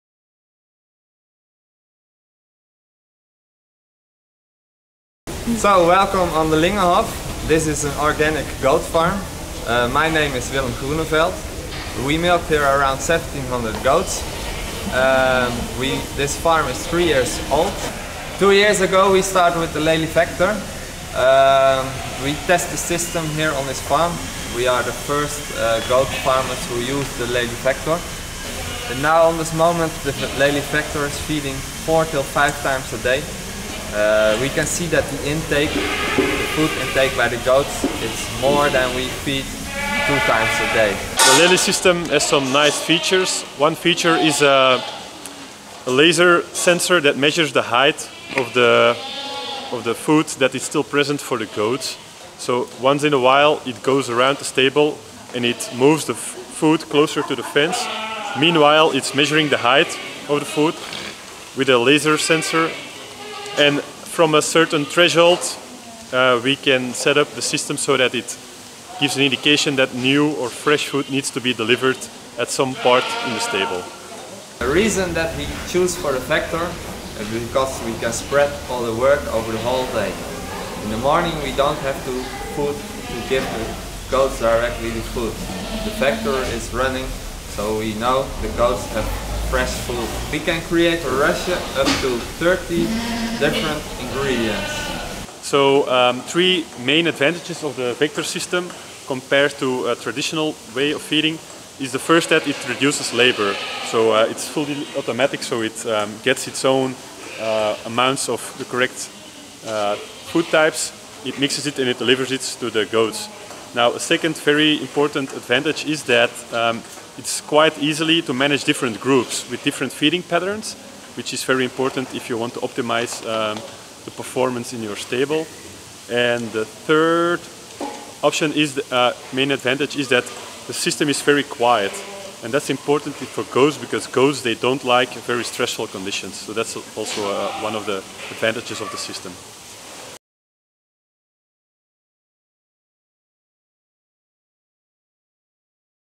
Automatic robotic feeder - lely vector - in use on a goat farm in Holland
Innovation for Agriculture visited De Lingehof farm in Holland, and learnt about how a goat farmer uses the lely automatic feeding robot, and the benefits it has brought to him.

This video was created for 4d4f and funded by European Horizon 2020.
To find out about more technologies in dairy farming visit the 4d4f